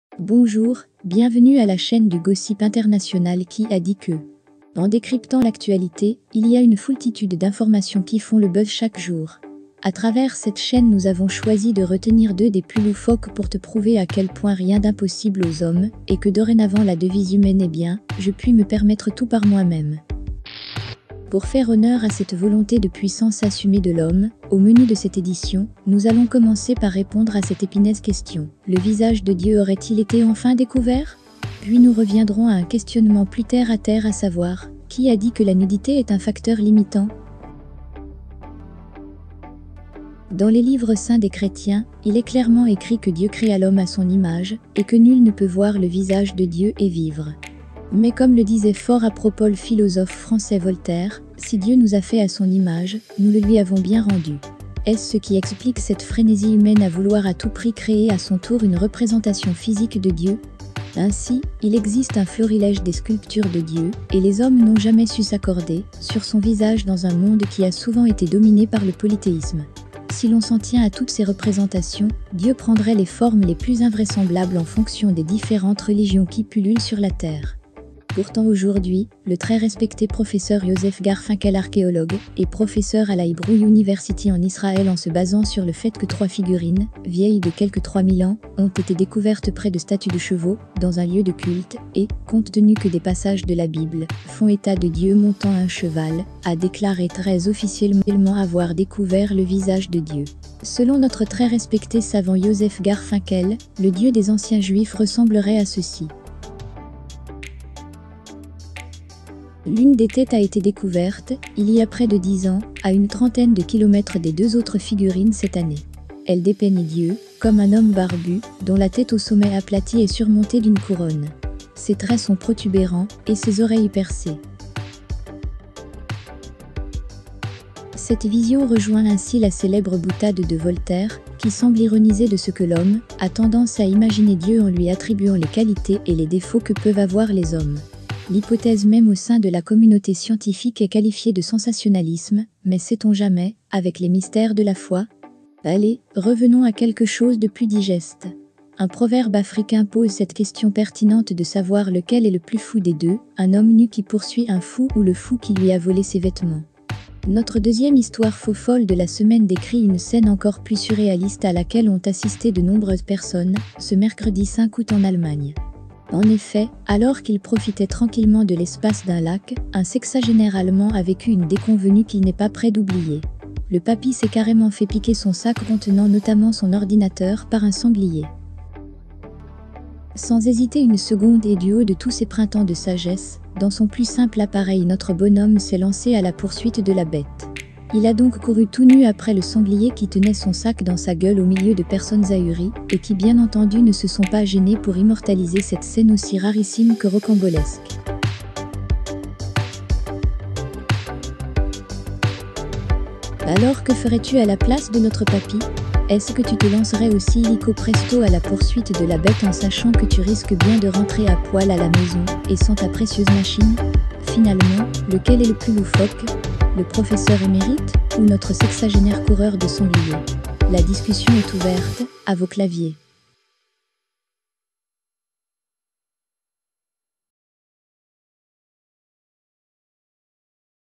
les sculptures du visage de dieu selon l'archéologie, un papy court après un sanglier...
Une découverte archéologique en lien avec la représentation de dieu, un sexagénaire qui court après un sanglier dans son appareil le plus simple! Découvrez comment et pourquoi dans cette vidéo!